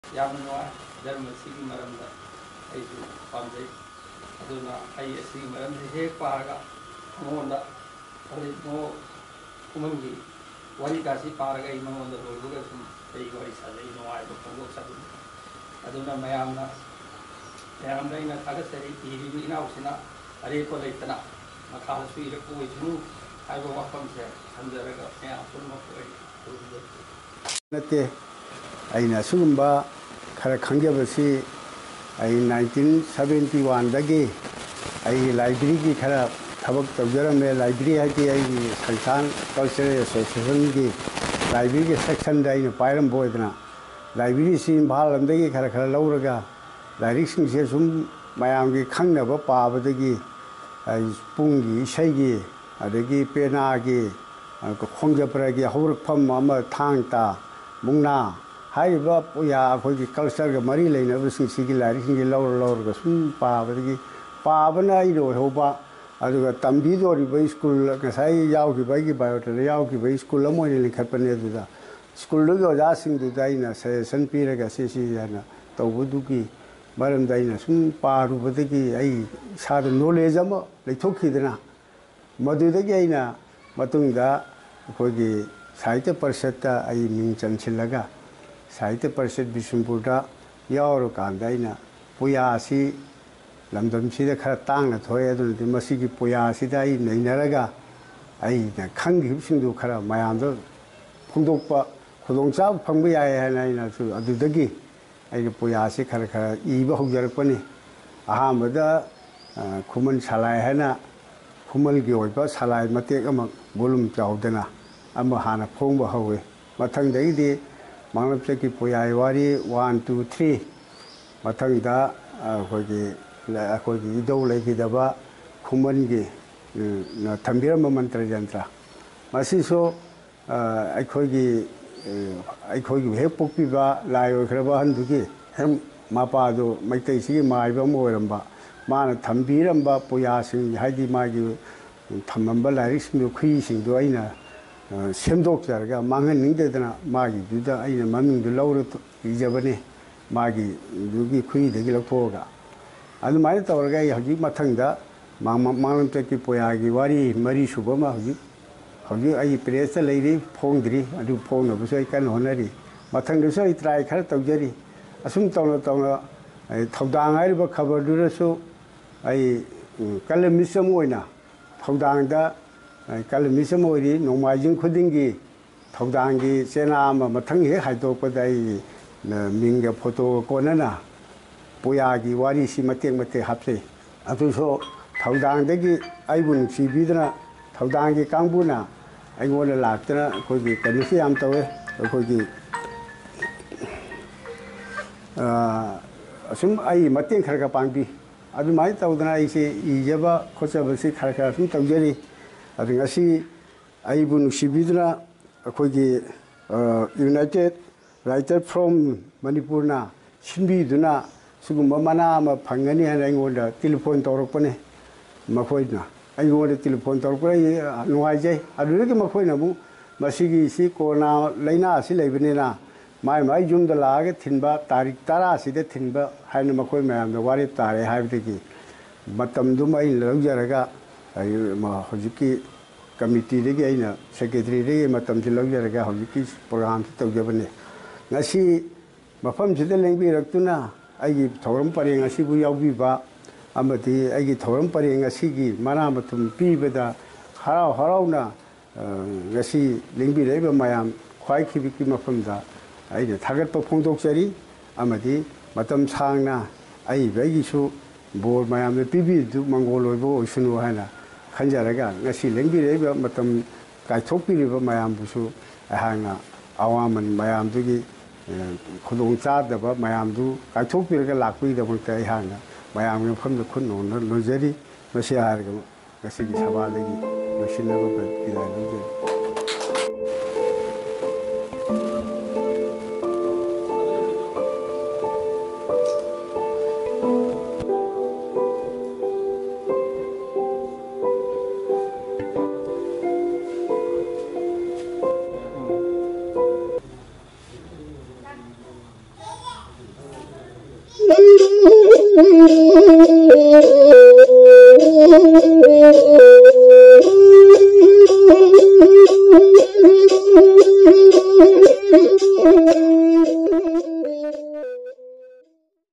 United Writers Forum Manipur Bishnupur distributes Literature Awards, 10/10/2020
United Writers Forum Manipur Bishnupur distributes Literature Awards, 10/10/2020 to selected Awardees, H. Itomcha & Manimohon Literature Award 2019 to Dr. N. B. Nongshaba of Ngaikhong Siphai, Sanjoykumar Memorial Award 2019 to Moirangthem Nilakumari Devi of Khongman, Mangang Apoy Literature Award 2019 to Laishram Ramjee Singh of Ningthoukhong. Because of Covid 19 Pandemic, all the Awards were handed over to their respective residences.